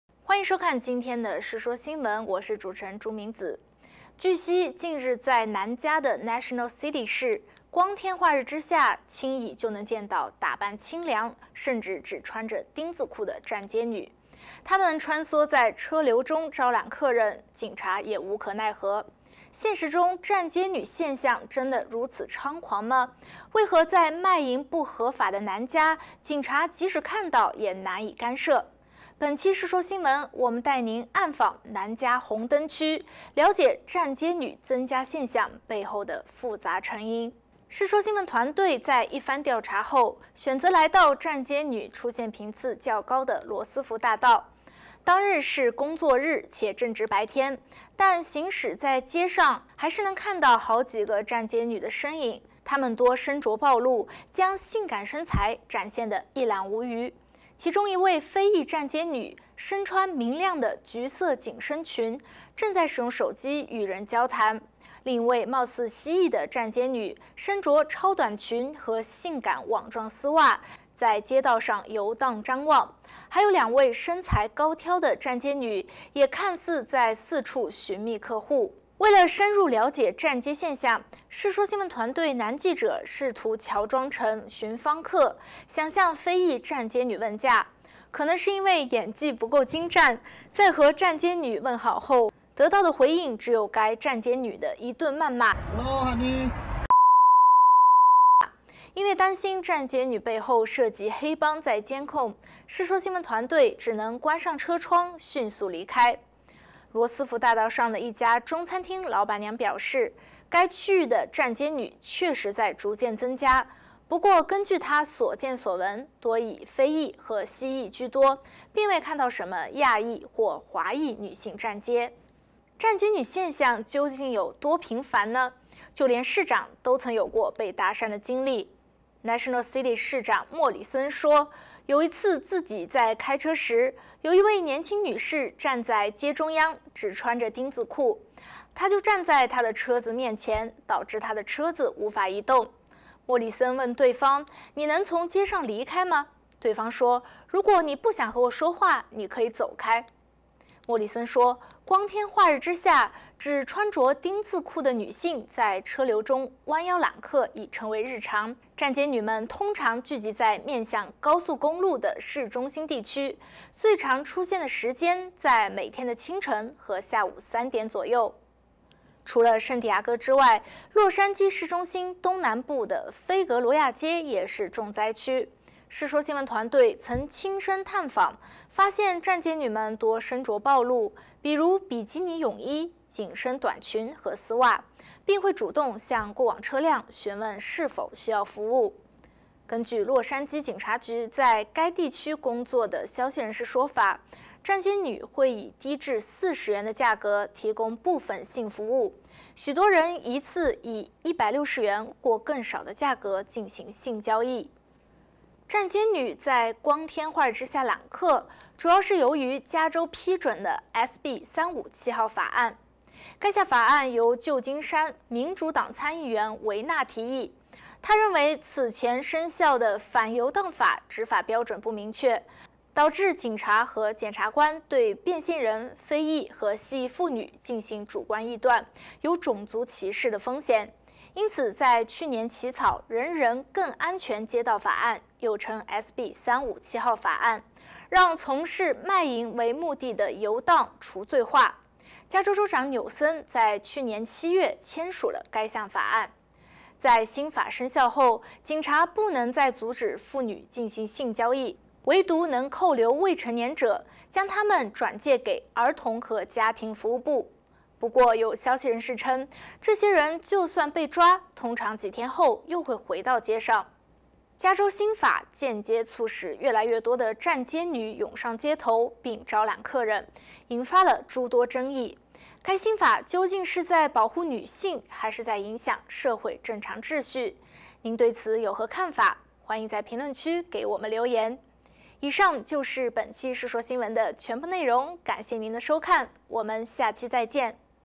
世說新聞╱暗訪南加紅燈區 站街女湧上街頭只因...
據悉，近日在南加的National City市，光天化日之下，輕易能見到打扮清涼、甚至只穿著丁字褲的站街女，她們穿梭在車流中招攬客人，警察也無可奈何。現實中站街女現象真的如此猖狂嗎？為何在賣淫不合法的南加，警察即便看到也難以干涉？本期世說新聞，我們帶您暗訪南加紅燈區，了解站街女增加現象背後的復雜成因。